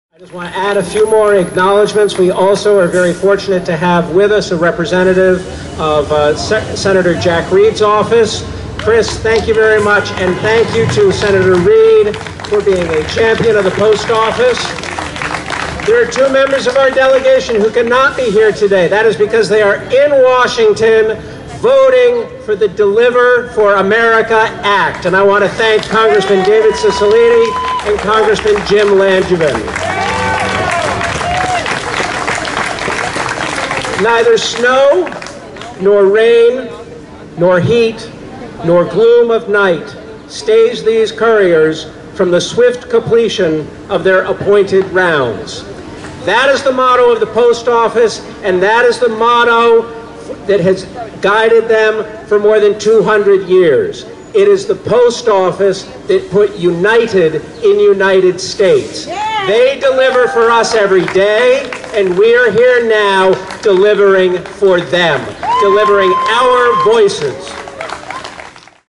Standing Strong: Rally to Save the Post Office
A gathering at a rally in support of the United States Postal Service took place on August 22, 2020. The event featured acknowledgments of key figures advocating for the Post Office, including a representative from Senator Jack Reed’s office, Chris. The rally highlighted the absence of two Rhode Island delegation members, Congressman David Cicilline and Congressman Jim Langevin, who were in Washington, D.C., voting for the Deliver for America Act. Participants emphasized the historic motto of the Post Office, “Neither snow nor rain nor heat nor gloom of night stays these couriers from the swift completion of their appointed rounds,” underscoring its vital role in uniting and serving the nation for over 200 years. The event served as a platform for community members to voice their support for the Postal Service.